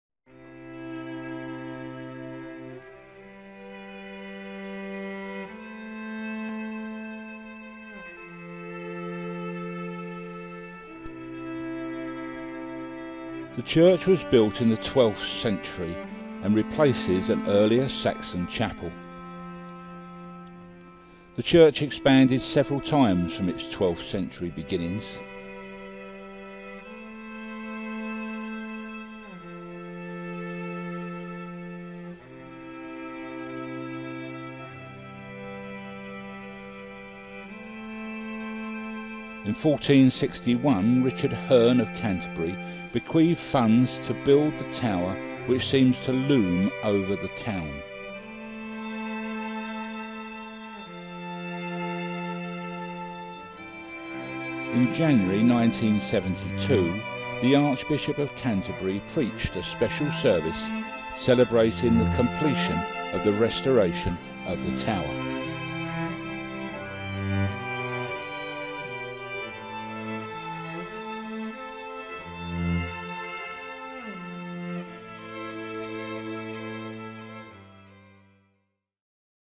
The history of Saint Mildred's church Tenterden
A brief history of Saint Mildred's church Tenterden Kent.